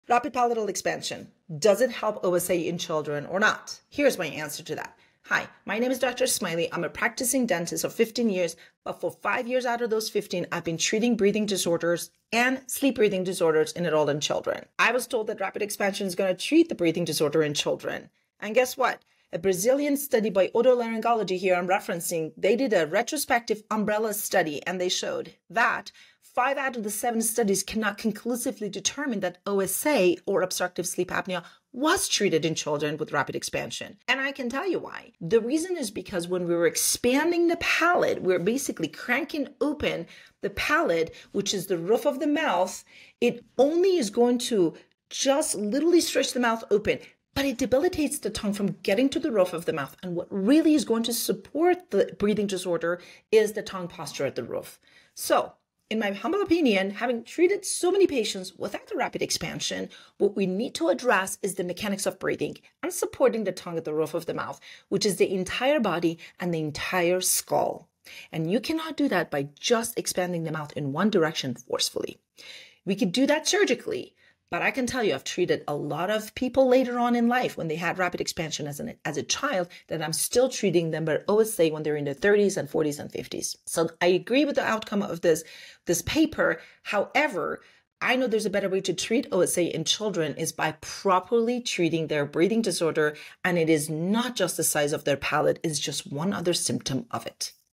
Rapid Palatal Expansion: does it actually help children with OSA?
A recent Brazilian review found that 5 out of 7 studies couldn’t confidently conclude that RPE treats obstructive sleep apnea in children.

As someone who’s spent years treating breathing disorders in both children and adults, I believe it’s time we look beyond quick-fix solutions.

The mechanics of breathing are complex—it's not just about widening the palate. It’s about tongue posture, muscle tone, facial growth patterns, and the why behind dysfunctional breathing in the first place.

Yes, RPE can help create space. But space alone doesn’t always solve the problem.

Let’s stop treating symptoms in isolation. Let’s start listening to the body as a whole. 🧠🌬️